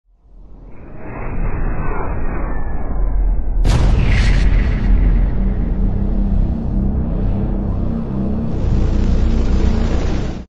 Spaceships
Oh my god, I’m scared! Why are they here 😱
I think this is a very cool blender animation that I made, hope you like it!

I made the modeling + animation in Blender
Edited with IMovie
Sounds are from Pixabay